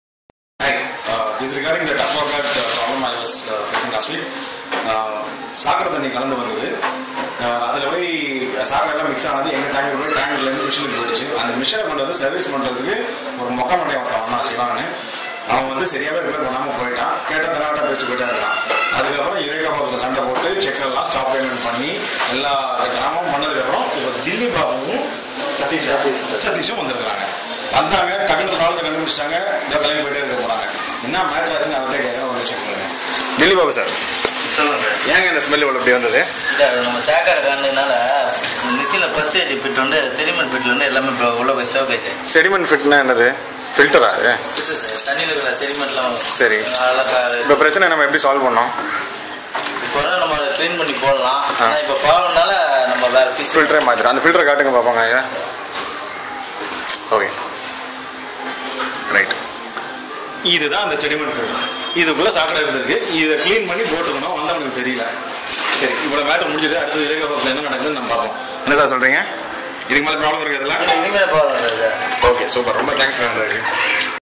Eureka Forbes problem solved...
At last two experts who came quickly identified and resolved the issue...
Thnx to Dilli Babu and Satish from Eureka Forbes for helping us with it...